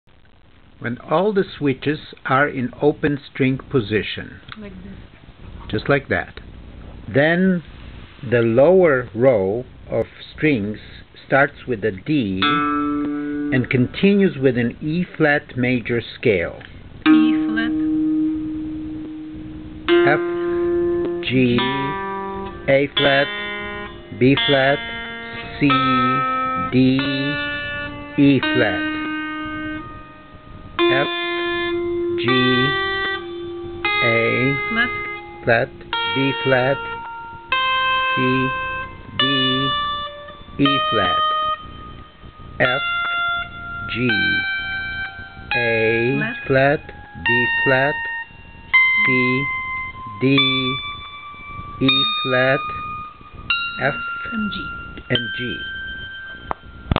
Bandura Tuning - Melody Strings
Melody Strings are tuned in Eb Major, while switches for changing the keys are in "open string" position.